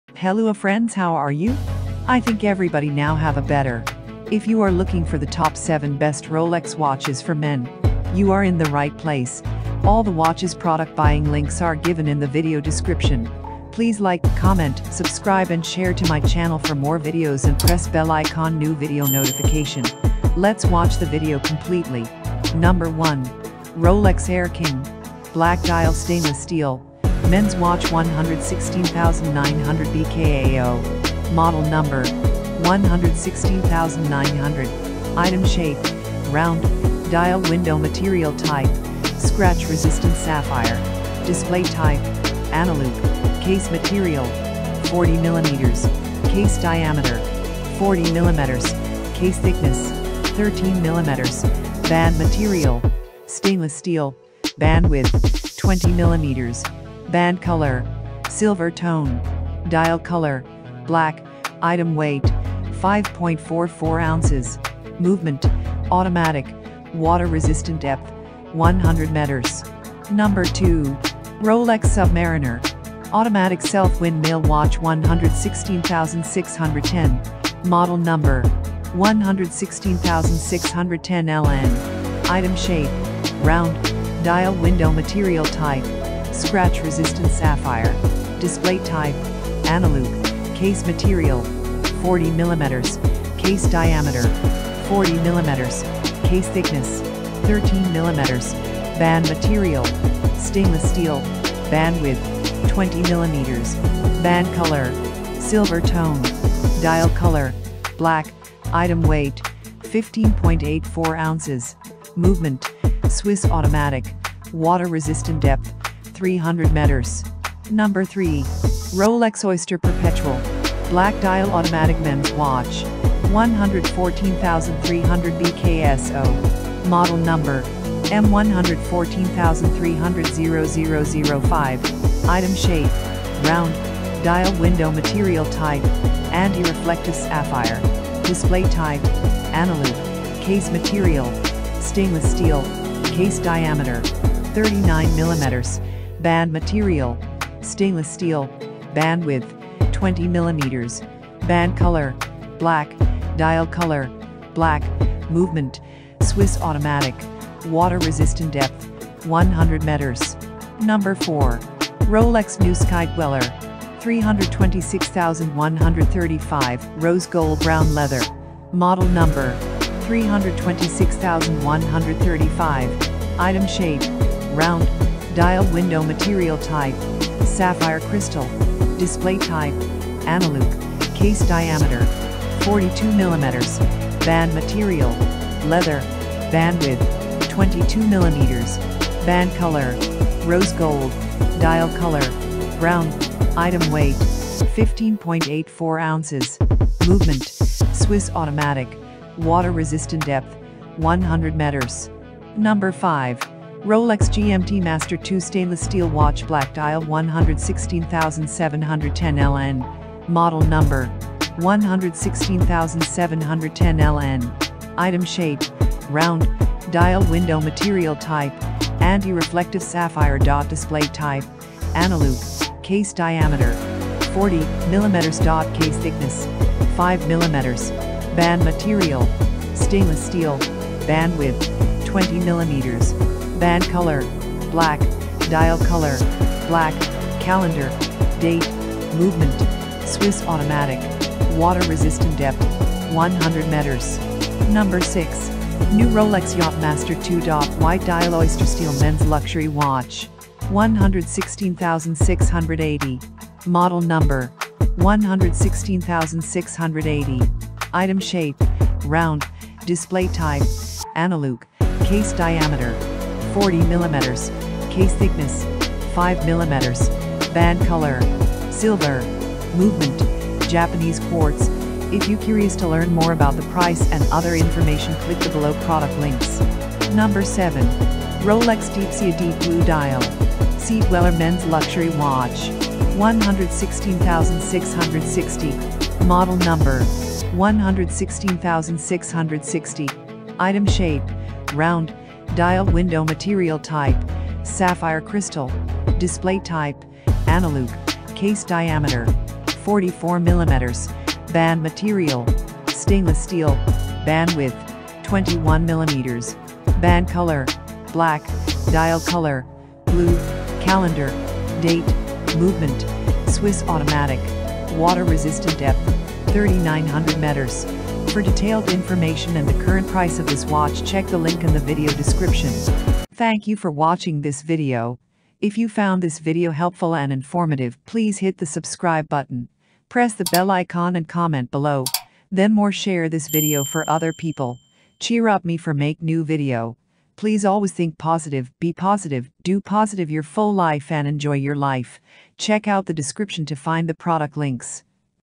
Best 7 Rolex watches for men |Best 7 Rolex Watches 2023|KKAhmed review amazon.
In this vedio I am going to share my experience for Best 7 Rolex watches for men |Best 7 Rolex Watches 2023|Best 7 Rolex watches for men 2022| KKAhmed review amazon.

1. Rolex Air King Black Dial Stainless Steel Mens Watch 116900BKAO
2. Rolex Submariner Automatic-self-Wind Male Watch 116610
3. Rolex Oyster Perpetual Black Dial Automatic Mens Watch 114300BKSO
4. Rolex New Sky-Dweller 326135 Rose Gold Brown Leather.
 5. Rolex GMT-Master II Stainless Steel Watch Black Dial 116710LN.
6. New Rolex Yacht-Master II White Dial Oystersteel Men's Luxury Watch 116680.
7. Rolex Deepsea Deep Blue Dial Sea-Dweller Men's Luxury Watch 116660.
If you found this video helpful and informative